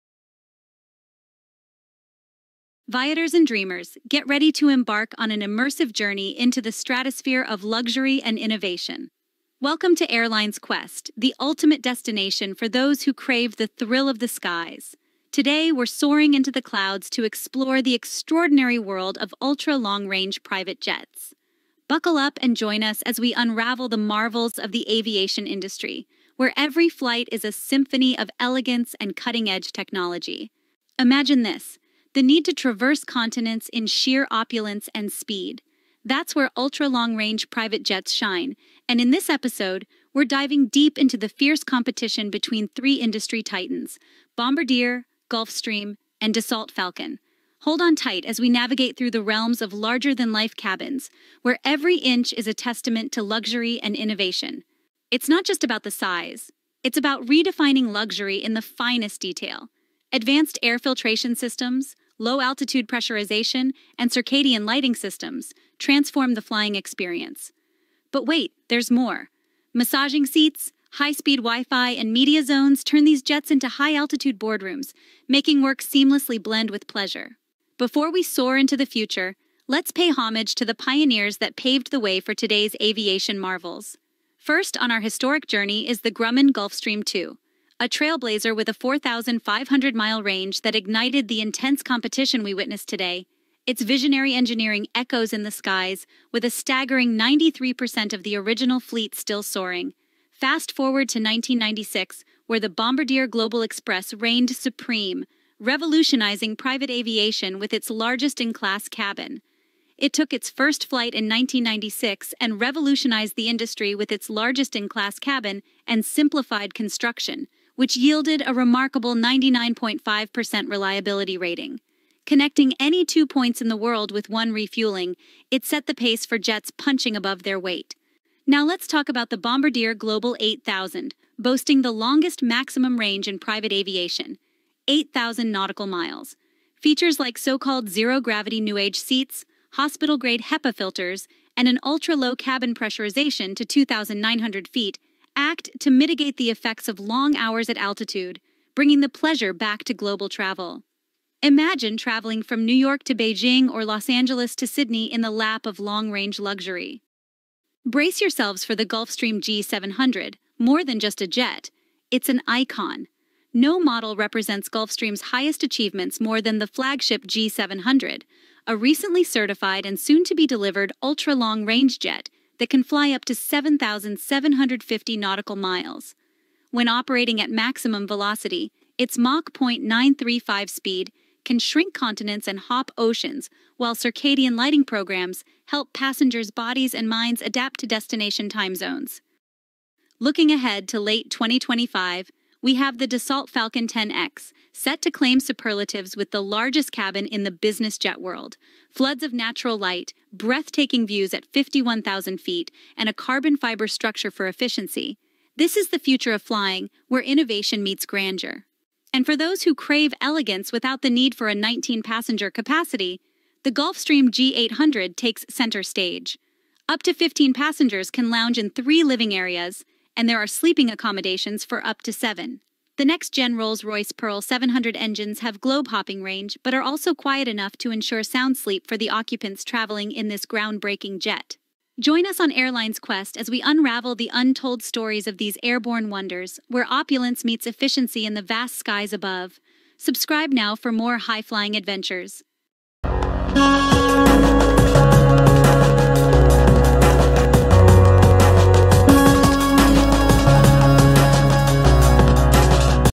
6 Best Ultra-Long Range Private Jets
Embark on a thrilling airborne adventure with Airlines Quest as we soar into the world of ultra-long-range private jets! In this episode, we delve into the fierce competition between aviation giants—Bombardier, Gulfstream, and Dassault Falcon. From historic pioneers like the Grumman Gulfstream II to the cutting-edge Gulfstream G800, every detail is a masterpiece of luxury and innovation.

- Bombardier Yotutube Channel
- Gulfstream Yotutube Channel
- Dassault Falcon Youtube Channel